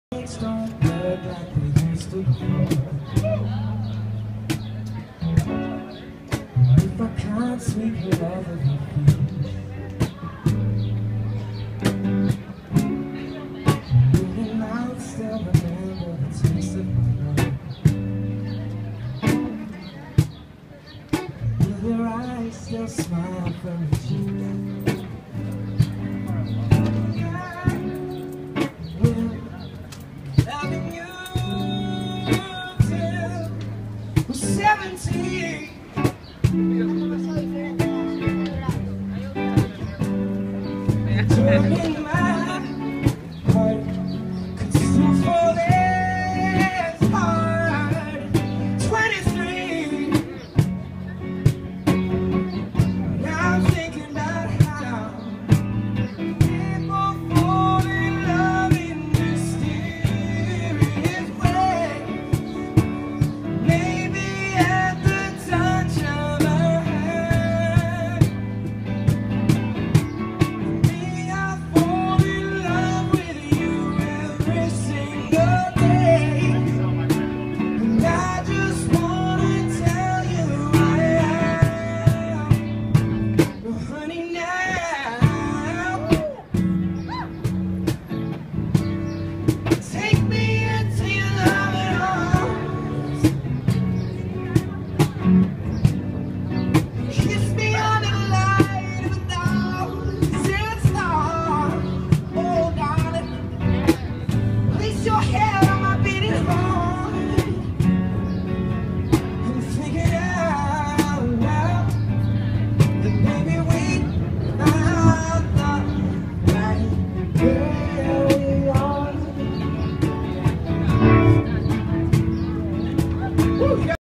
Newbury St, Boston, MA
The first really warm day in Boston. April 12, 2015.